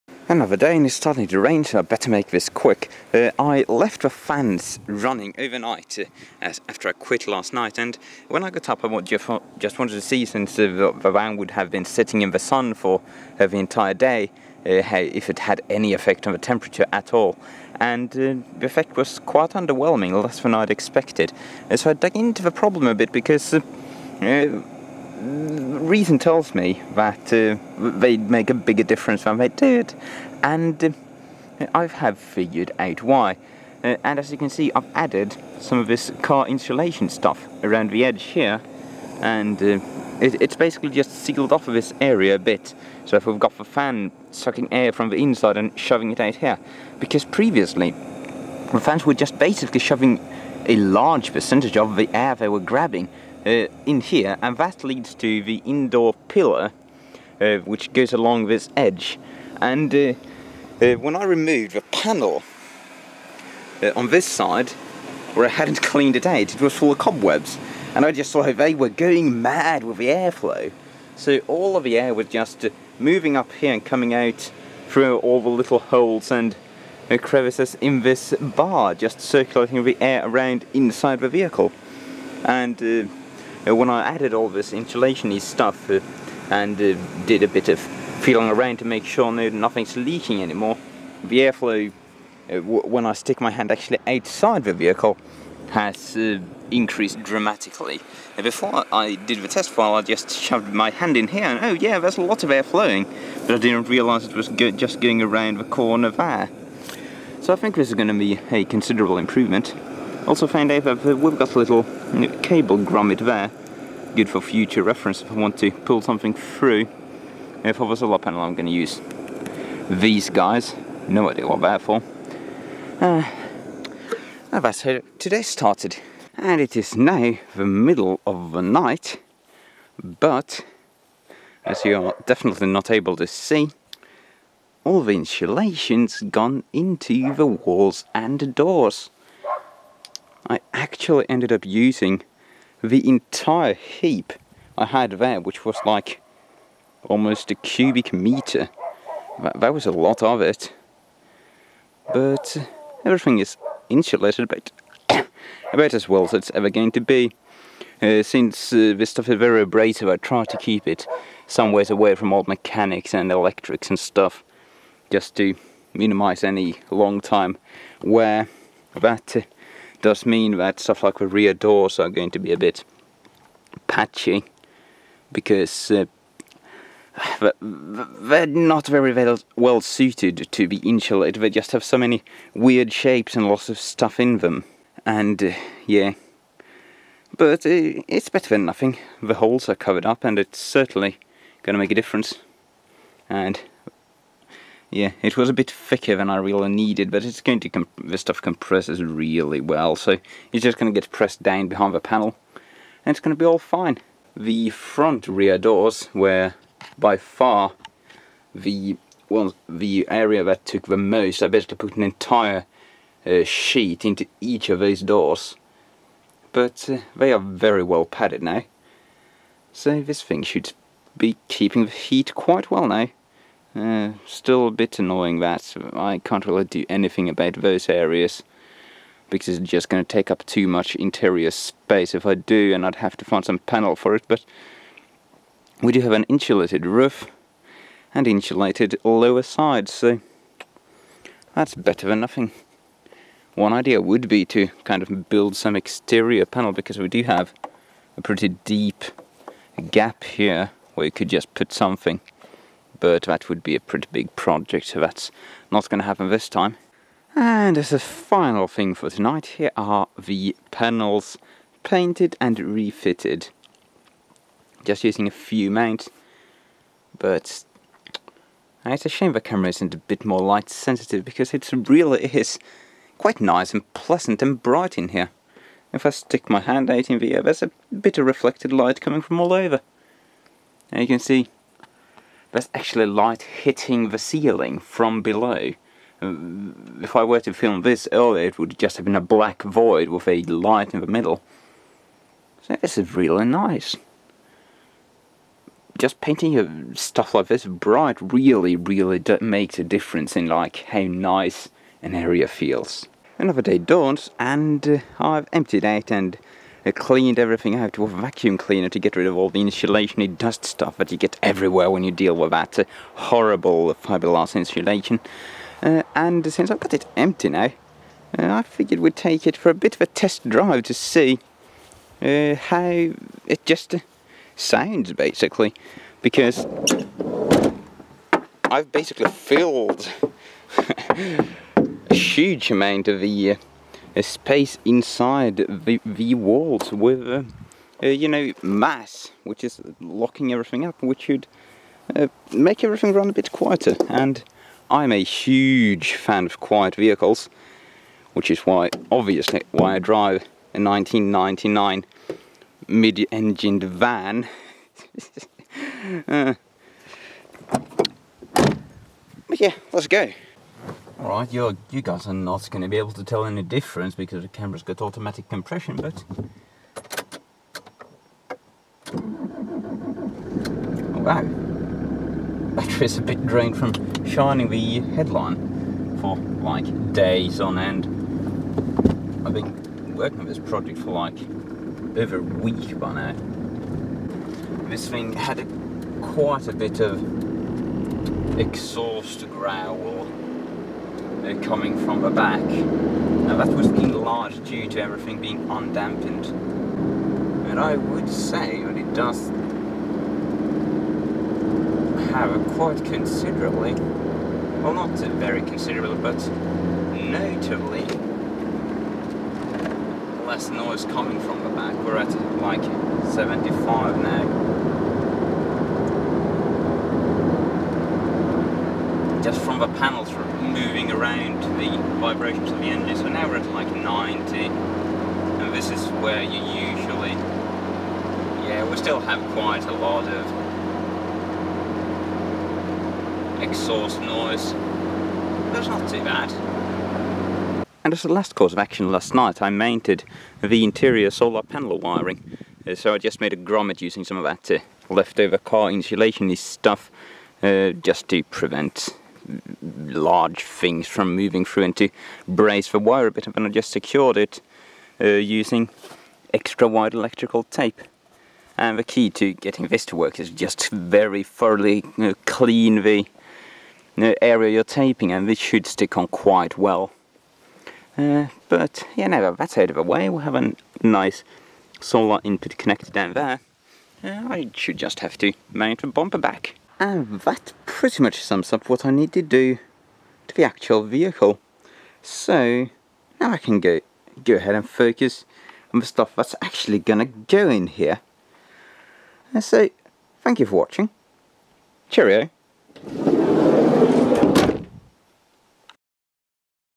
Van camper! Build log #4 - Insulation & walls
Installing insulation and interior panels, as well as some wiring for the fans and solar panel.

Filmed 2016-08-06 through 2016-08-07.

A big thank you goes out to a good friend of mine, who generously donated all the insulation for this project, as well as let me borrow a proper insulation knife, saving me well over 50 € and a lot of labour.

Testing after this video was shot confirms that the insulation in the walls is VERY effective, and even the thin soundproofing material does a surprisingly good job at keeping the cold out. The (by FAR) worst heat leak left after this video is the windows.